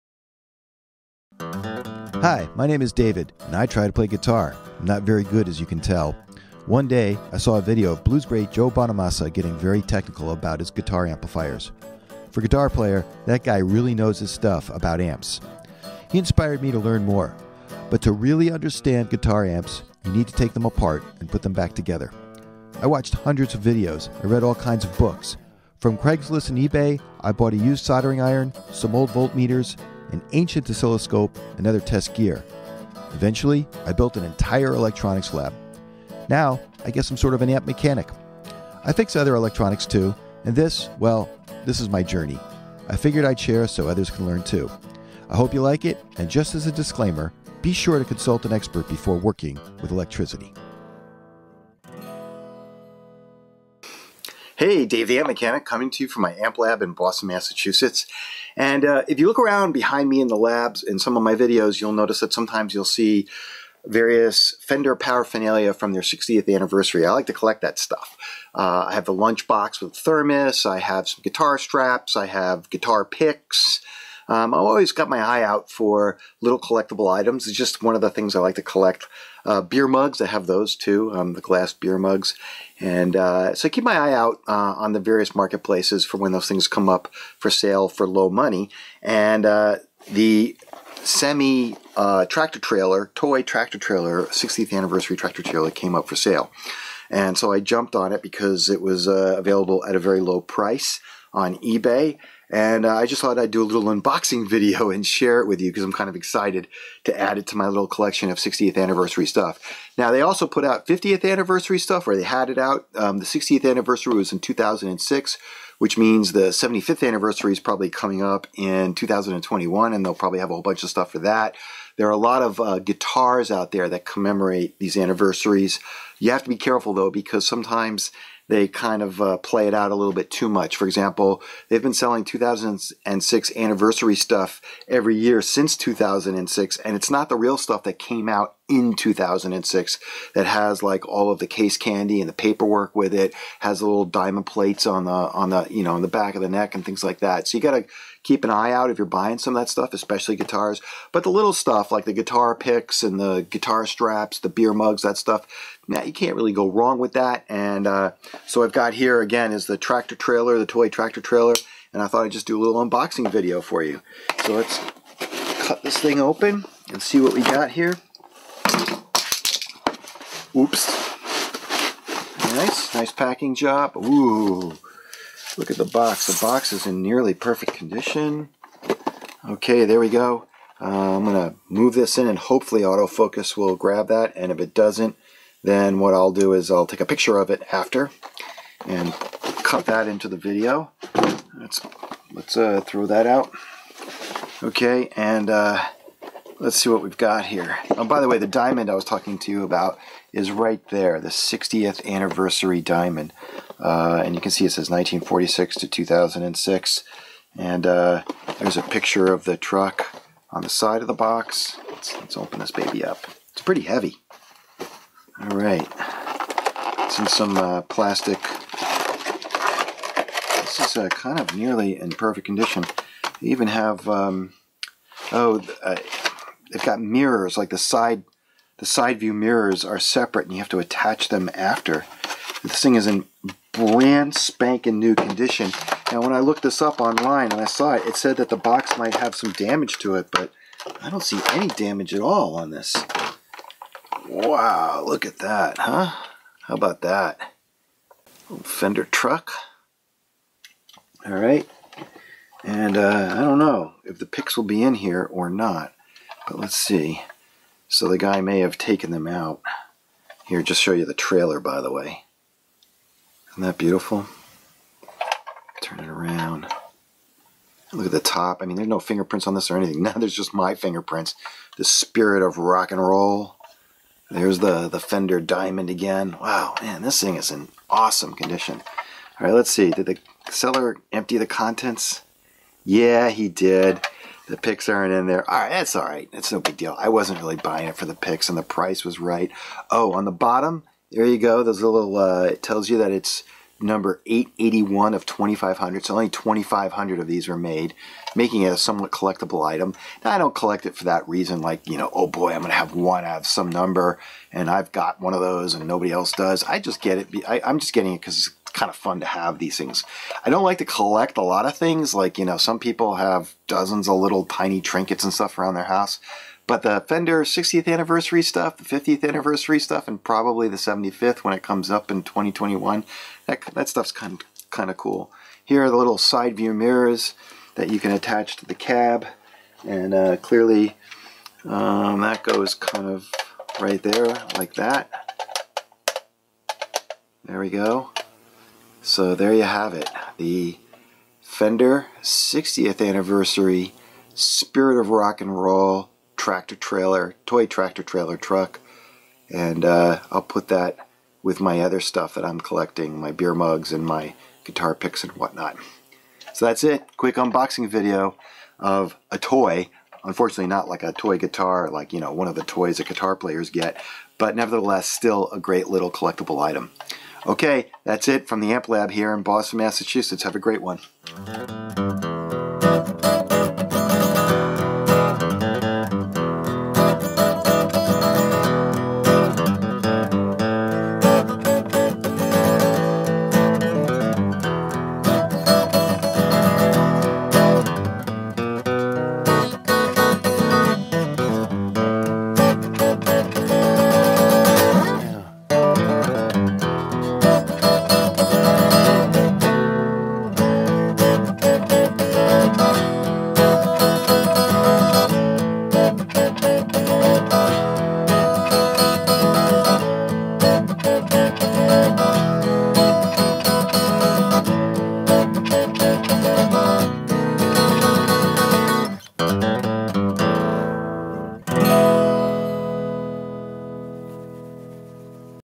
Unboxing of Fender 60th Anniversary Tractor Trailer Truck (AmpMechanic #015)
OK, not exactly a repair video from the AmpMechanic's amp lab and I try not to be a collector of a million little trinkets that my descendants will one day have to unload for $5 at a garage sale. But I really like the Fender 60th (Diamond) Anniversary commemorative stuff and I keep my eye out for when there's a good deal on one of the items. This time, I acquired the commemorative tractor trailer for low money and so, my collection expands a little bit more. If you decide to collect the same, just be sure not to buy the so-called 60th anniversary stuff (especially guitars) that actually came after 2006. You can tell by the symbol. If it's really 2006, it has the diamond on it. If it was the "Fender is just milking it" stuff, the symbol is different (a ring of leaves around a guitar).